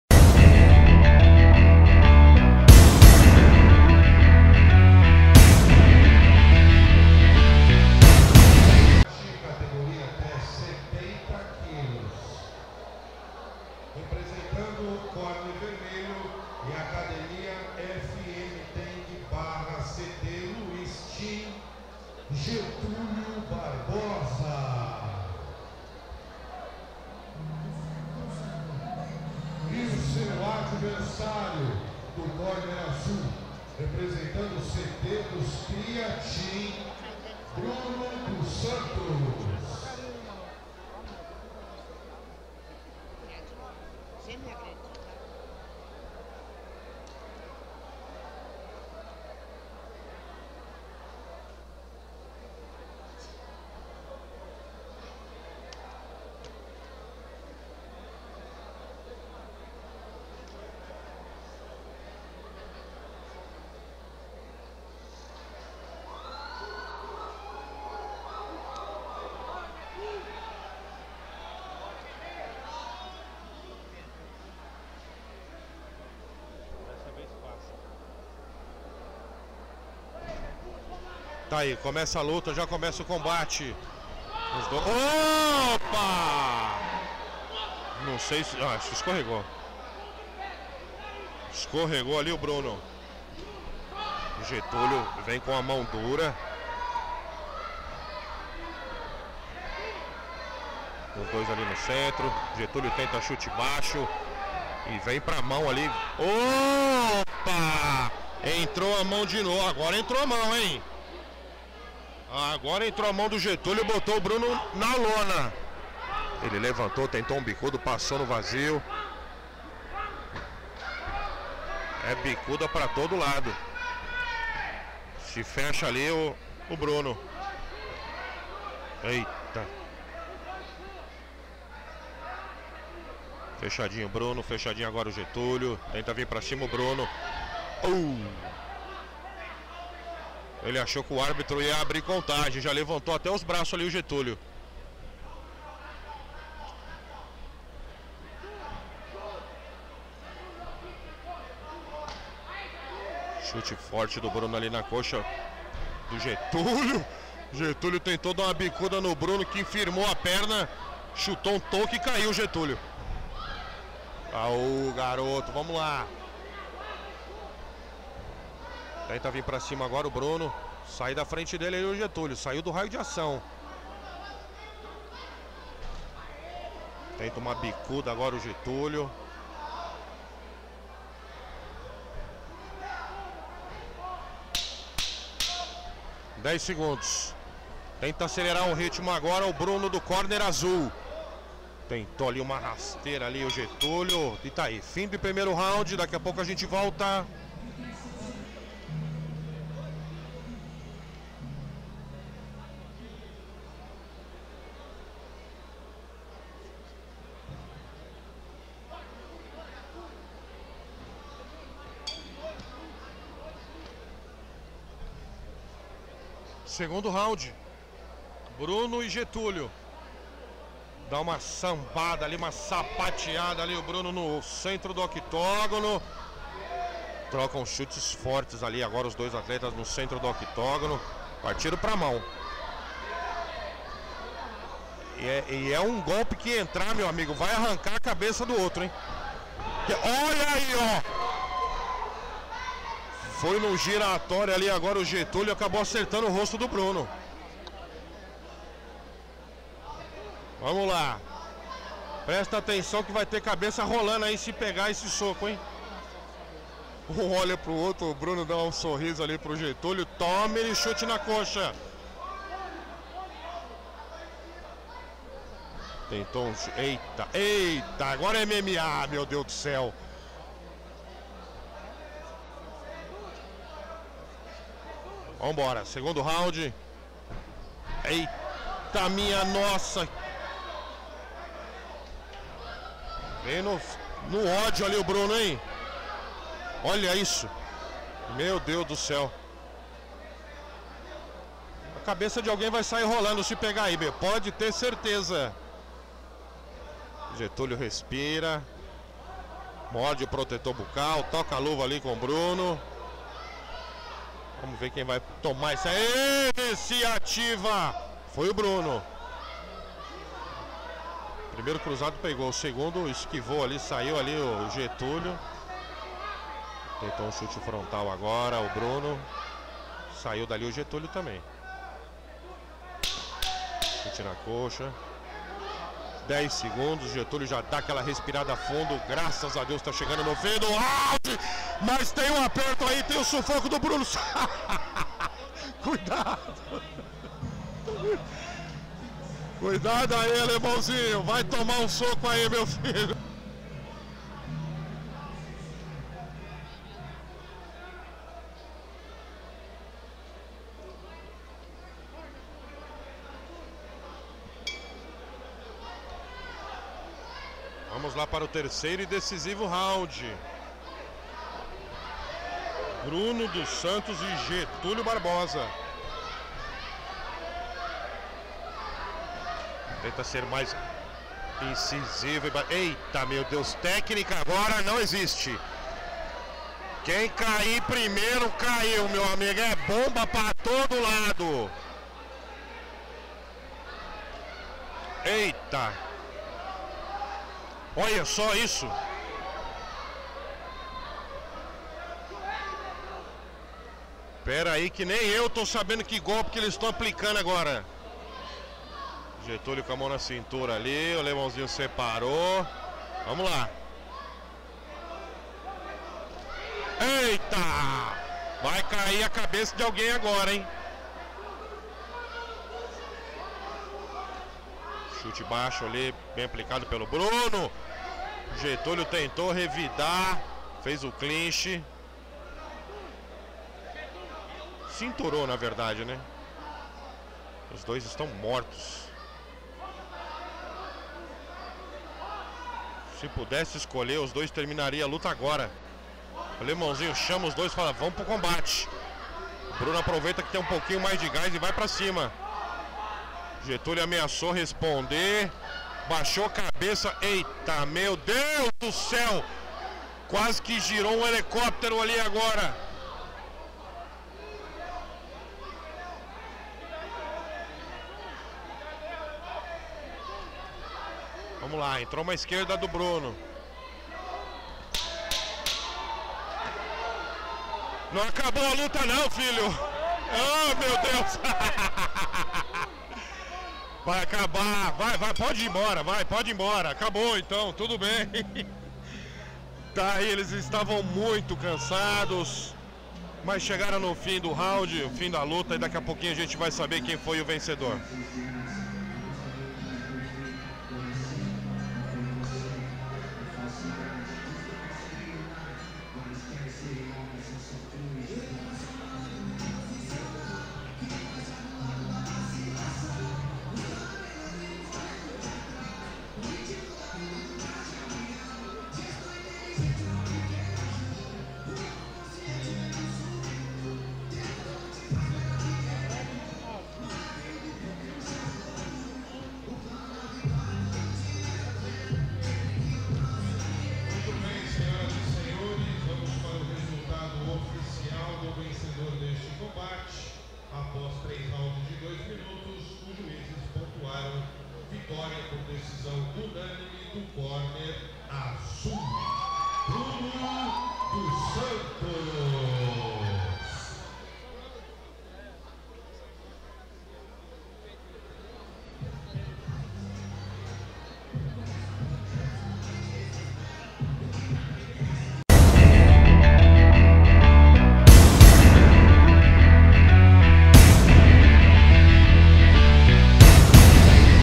Getulio vs Bruno - X1 Kombat 10
Em combate de MuayThai amador até 70kg, Bruno dos Santos (CT dos Criah Team) vecneu Getulio Jefferson (FMTEMG/CT Luiz Team) por pontos.